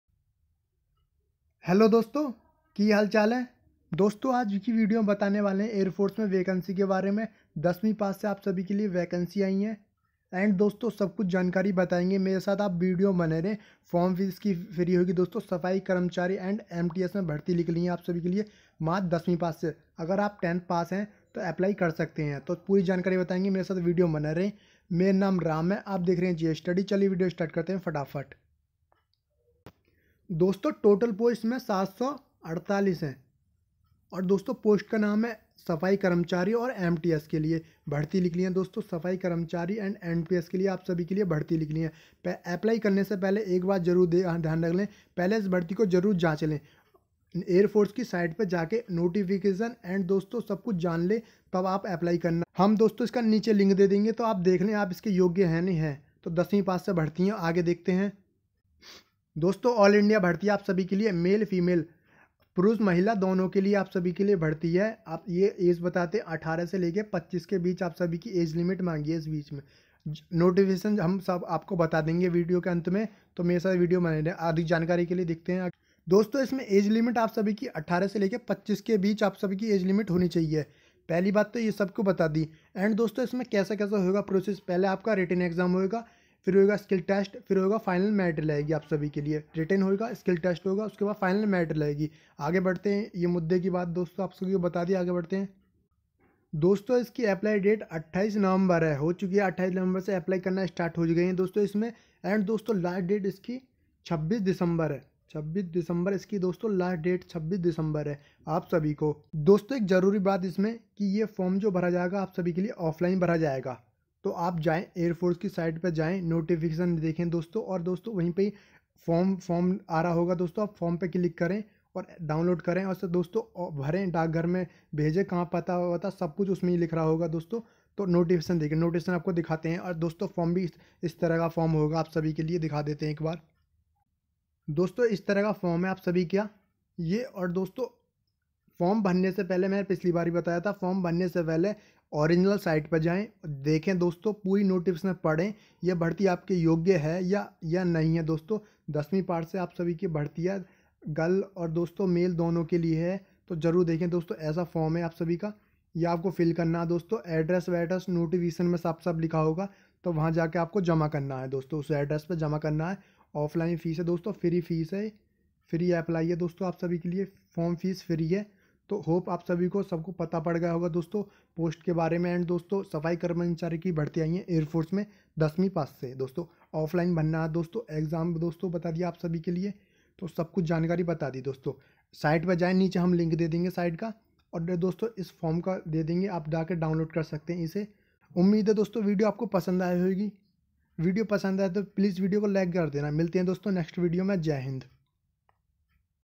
Indian AirForce Bharti 2020 | 10th Pass Vacancy | All India | Air Force Requirements 2020
सम्पूर्ण जानकारी के लिए Air Force ki Official Website पर Click करें

यदि आप इस भर्ती योग्य है तो फार्म भरै।।
 Air Force Requirements 2020 | air Force vacancy 10th pass | MTS | सफाई कर्मचारी | all India Jobs